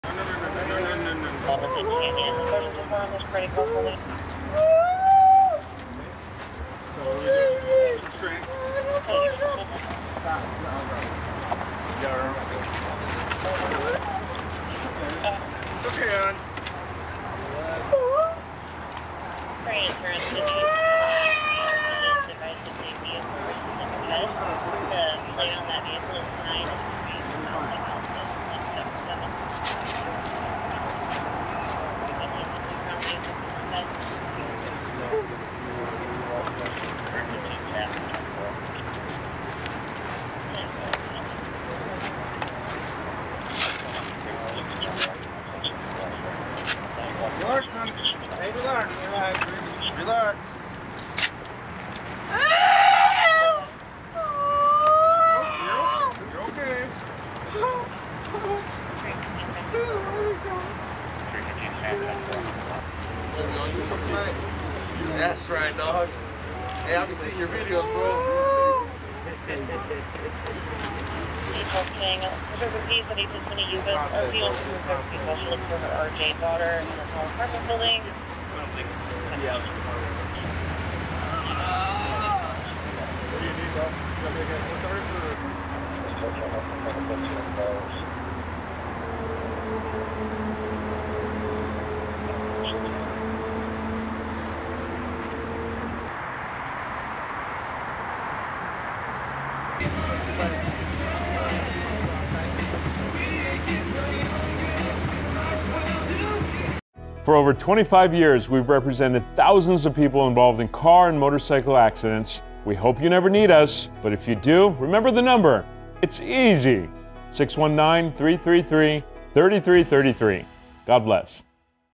“Don’t Drive On Tracks” Fiasco
On 5/6/23 at approximately 11:00 PM, Trolley Security Called San Diego Police stated someone was driving on the trolley tracks. Trolley enforcement state the driver hit several signs, including a “Do not drive on tracks” sign and fled the area. Roughly 10 minutes later, she was spotted near 17th & Imperial trying to drive wrong way on the freeway on ramp. Several bystanders got the suspect out of the vehicle and called Police. The female was transported to a nearby hospital for evaluation.